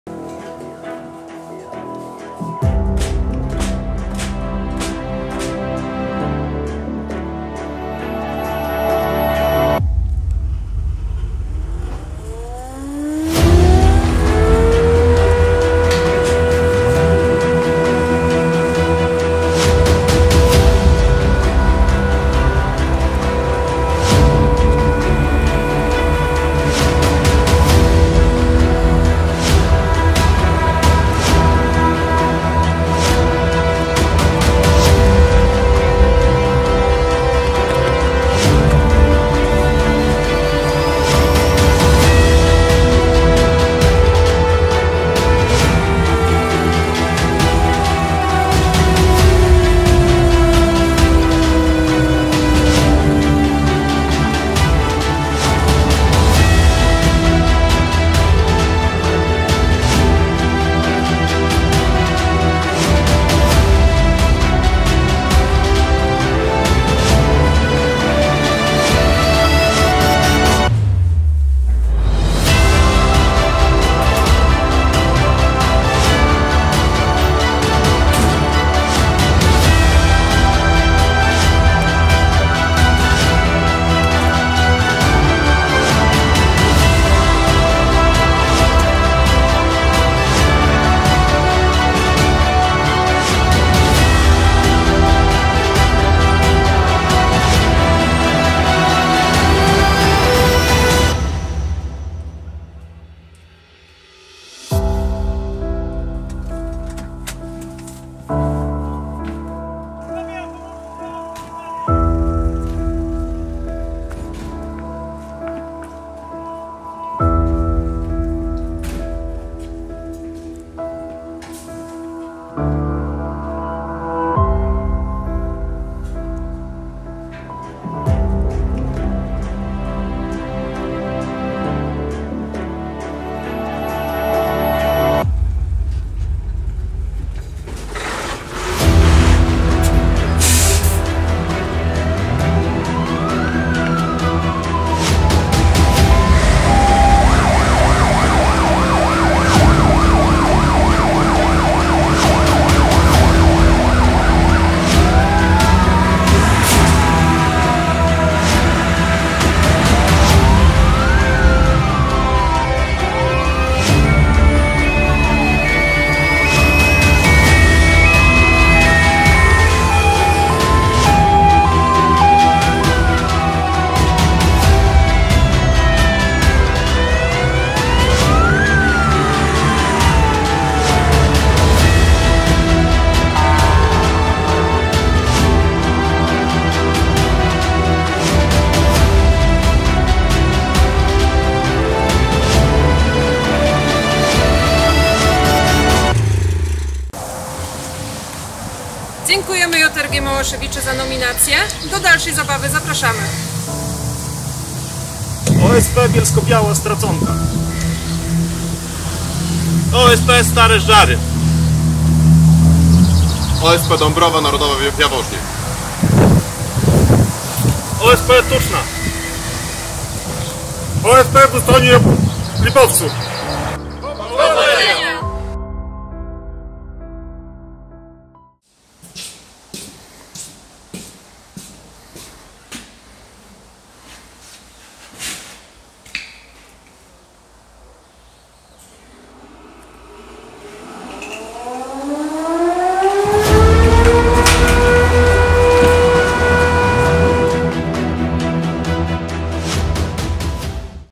AkcjaWyjazd OSP KSRG Zalesie
JRG Małaszewicze dziękujemy za nominację w
wyzwanie podjęte❗💪

Potrzebna jest pomoc dla ❤️Ani❤️, która walczy z SMA (rdzeniowy zanik mięśni)❗❗❗
Z racji że Ochotnicza Straż Pożarna słynie z pomagania bliźnim, włączamy się w nowe wyzwanie pod nazwą
Jest to akcja charytatywna, polegająca na nagraniu filmiku, w którym pokazane jest jak członkowie OSP, bądź innych służb mundurowych zbierają się do wyjazdu alarmowego oraz wpłaceniu przez nominowanych minimum 10zł na rzecz ❤️Małej Ani❤️. Czas na wykonanie zadania 48h od otrzymania nominacji.

Nominujemy:
🚒 Ochotnicza Straż Pożarna Bielsko-Biała Straconka
🚒 OSP Stare Żdżary
🚒 OSP Dąbrowa Narodowa
🚒 OSP Tuczna
🚒 OSP w Ustroniu Lipowcu

Losy dziewczynki z nierównej walki z chorobą możecie śledzić na stronie
Do dzieła! Pomagamy, udostępniamy. Niech się niesie !

Dziękujemy za realizację i montaż filmu Paweł Bielecki, super robota!!!